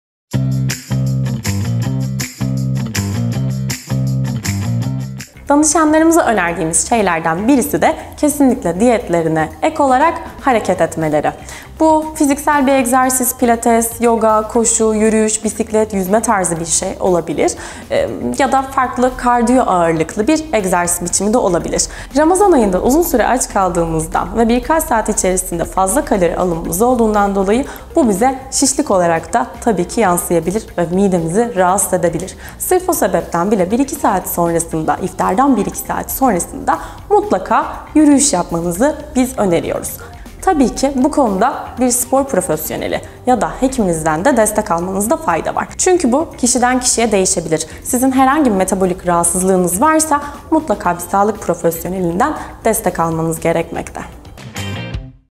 Ramazanda Spor Nasıl Yapılmalı? - Dyt. Cemre Balkan
Medicana International Samsun Hastanesi Beslenme ve Diyet bölümünden Dyt. Cemre Balkan ''Ramazanda Spor Nasıl Yapılmalı?'' sorusunu sizler için yanıtladı.